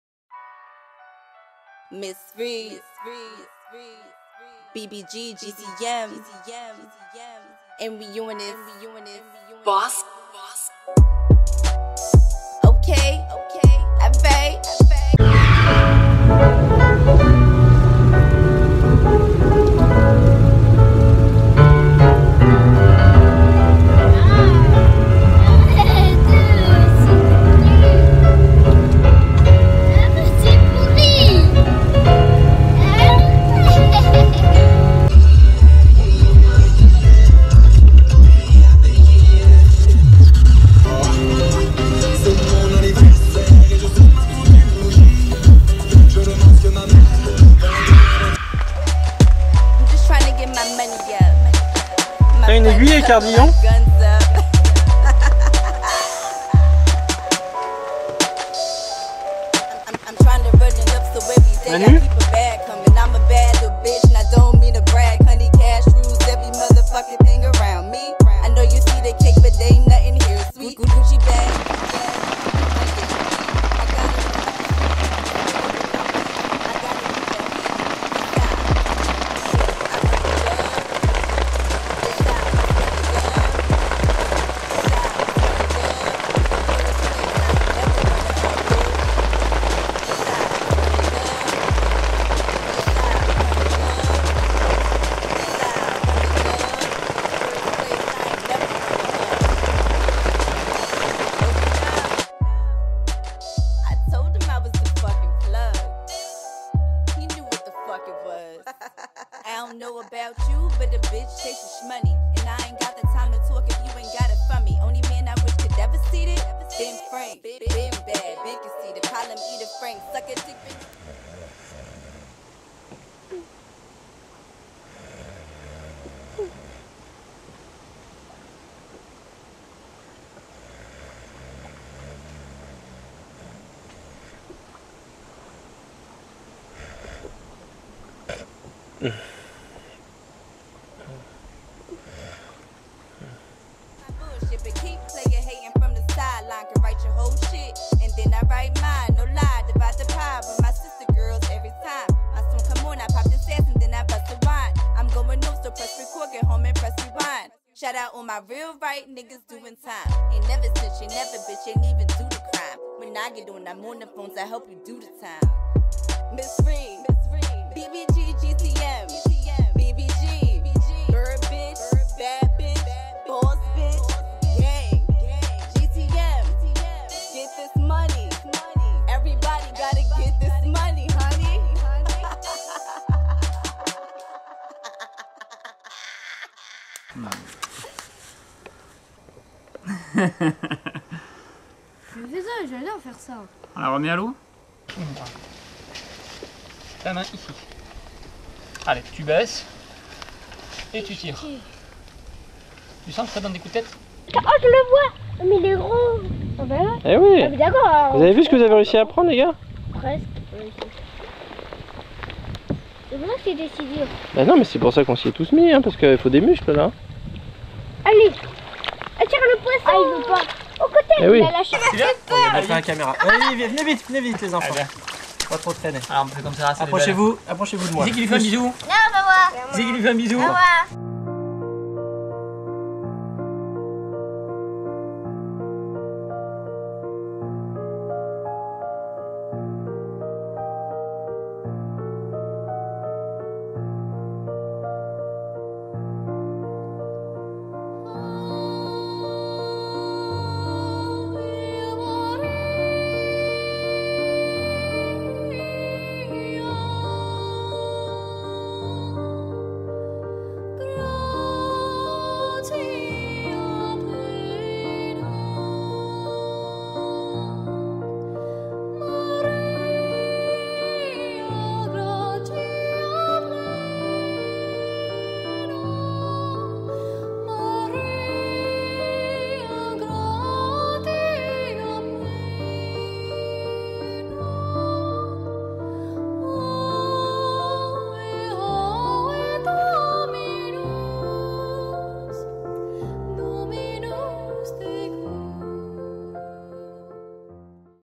Bouxier : un jeu d'enfants (1.la pluie)
C'est à l'occasion d'un reportage à paraître à la fin de l'année pour la revue Média Carpe, que l'équipe de Numa Fishing, augmentée pour l'occasion d'Enéa et Nathanaël, sont partis une semaine sur le domaine de Bouxier, à la rencontre des carpes miroirs, communes ou amour du plan d'eau. L'occasion d'un film souvenir autour de la transmission.